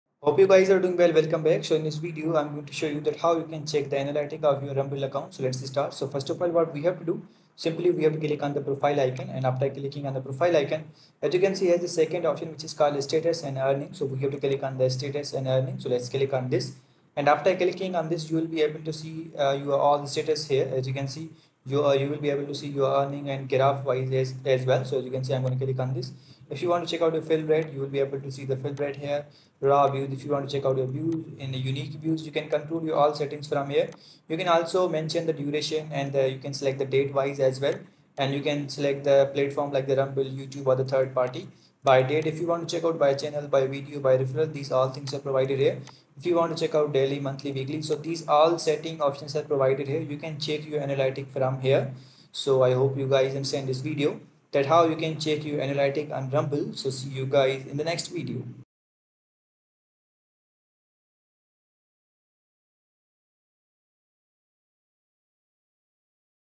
How to Check Analytics of Rumble Account [easy]
Checking the analytics of your Rumble account is simple. First, log in to your Rumble account. Once you're logged in, navigate to the "My Content" section, usually located in the main menu or dashboard. In the My Content section, you'll see an overview of your videos.

To access detailed analytics, click on a specific video for which you want to view the analytics. This will take you to the video’s analytics page, where you can see detailed statistics such as views, engagement, revenue, and other performance metrics. Analyze the data to understand your audience and video performance better. This will help you make informed decisions to improve your content strategy on Rumble.